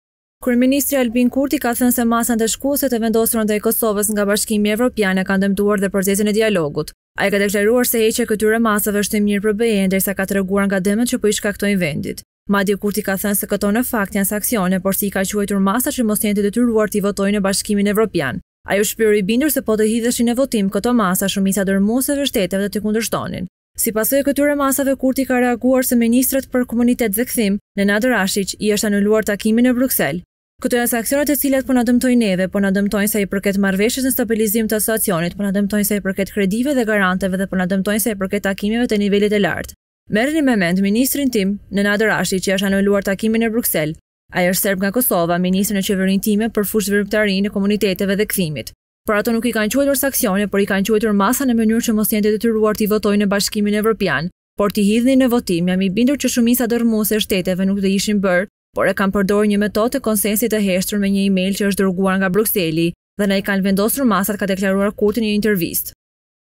Kurti për masat ndëshkuese/ Janë sanksione, i quajtën masa që të mos detyrohen t’i votojnë në BE
Kryeministri Albin Kurti ka thënë se masat ndëshkuese të vendosura ndaj Kosovës nga Bashkimi Evropian, e kanë dëmtuar edhe procesin e dialogut. Ai ka deklaruar se heqja e këtyre masave është mirë edhe për BE’në, derisa ka treguar për dëmet që po i shkaktojnë vendit.
Madje, Kurti ka thënë se këto në fakt janë sanksione, por se i kanë quajtur masa që të mos jenë të detyruar t’i votojnë në Bashkimin Evropian. Ai është shprehur i bindur se po të hidheshin në votim këto masa, shumica dërmuese e shteteve do t’i kundërshtonin.
Si pasojë e këtyre masave, Kurti ka treguar se Ministrit për Komunitete dhe Kthim, Nenad Rashiq, i ishte anuluar takimi në Bruksel.
“Këto janë sanksione të cilat po na dëmtojnë neve, po na dëmtojnë sa i përket marrëveshjes se Stabilizim Asocimit, po na dëmtojnë sa i përket kredive dhe granteve dhe po na dëmtojnë sa i përket takimeve të nivelit të lartë. Merreni me mend, ministrit tim Nenad Rashiq i është anuluar takimi në Bruksel. Ai është serb nga Kosova, ministër në qeverinë time për fushveprimtarinë e komuniteteve dhe kthimit. Por, ata nuk i kanë quajtur sanksione, por i kanë quajtur masa në mënyrë që mos të jenë të detyruar ti votojnë në Bashkimin Evropian. Po t’i hidhnin në votim, jam i bindur që shumica dërmuese e shteteve nuk do të ishin për, por e kanë përdorur një metodë të konsensusit të heshtur me një email që është dërguar nga Brukseli dhe na i kanë vendosur masat”, ka deklaruar Kurti, në një intervistë për Deutsche Welle.

𝐀𝐮𝐭𝐨𝐫 : NTV